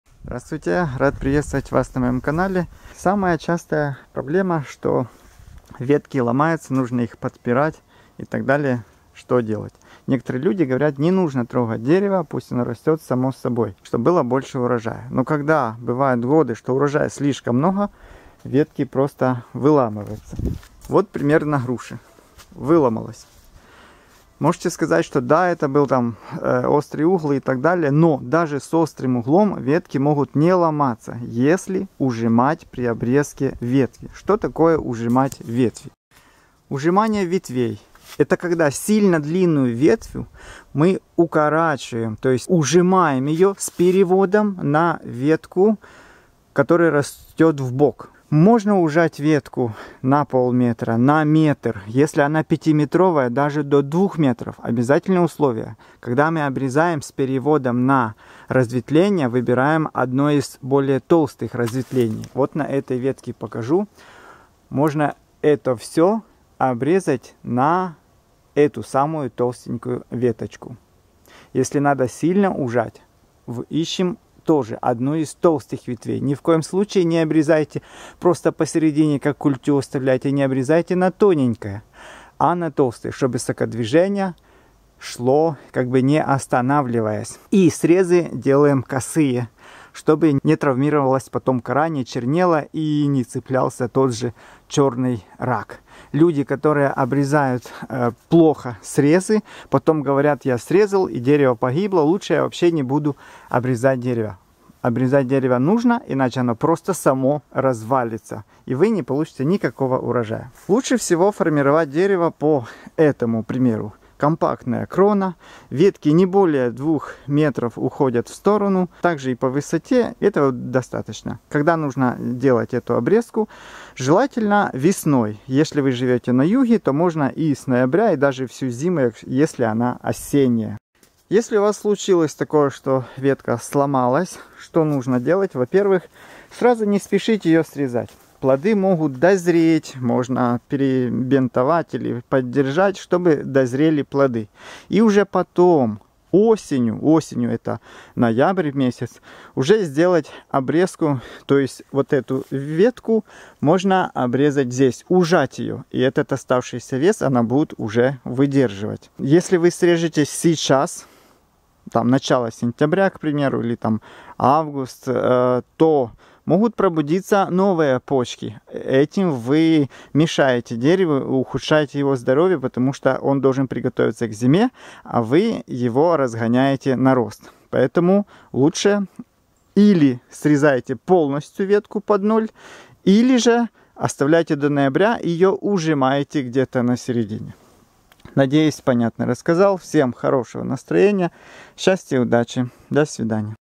Что делать если сломалась ветка на яблоне.
виталий7соток Ветка или ужимается сильно в нояьре, или отпиливается вовсе. Срез закрашивается краской для пола или препаратом Максим. Можно Бордосской смесью.